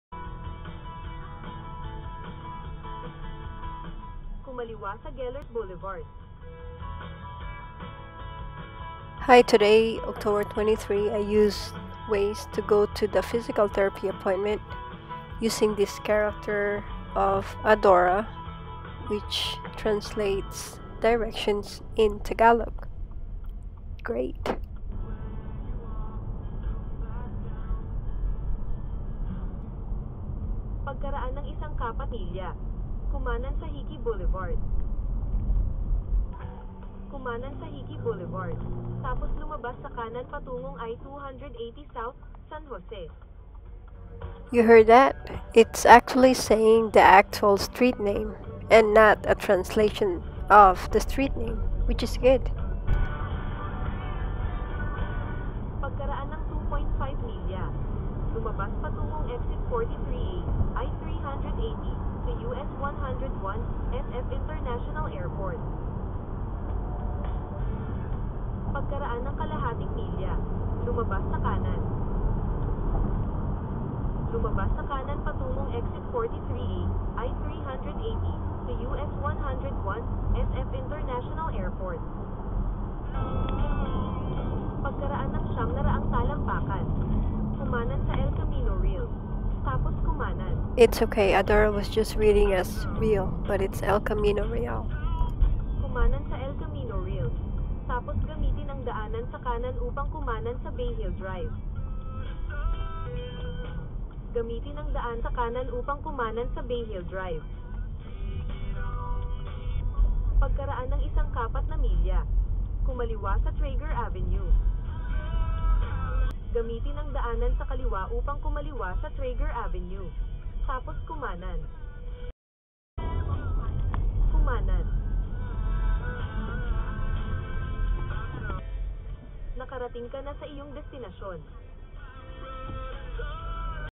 Waze app using Adora's voice- Filipino
10/23/2020, for my drive to my Physical appointment today, I tried to use waze using the character of Adora, which is a Filipino character on Waze app for driving directions. Im impressed on how it is translated in Tagalog but not in deep words, street names and exit were said as is. It's awesome. Good job waze programmers, engineers and technicians.